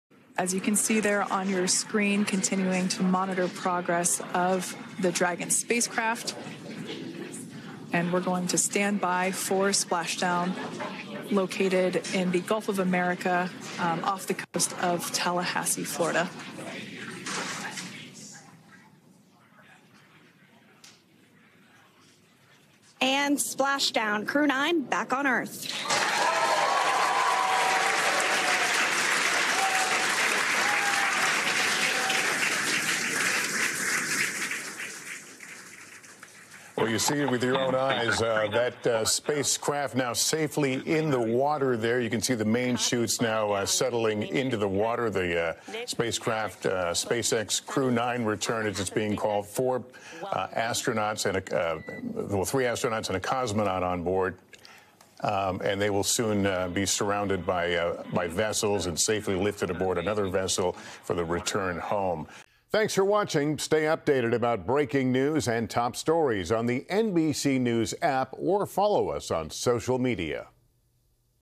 BREAKING: NASA astronauts splash down after nine months in space
NASA's Crew-9 astronauts splashed down, landing off Tallahassee, Florida, in their SpaceX Dragon spacecraft after two of the four crew members, Suni Williams and Butch Wilmore, spent nine unexpected months in space.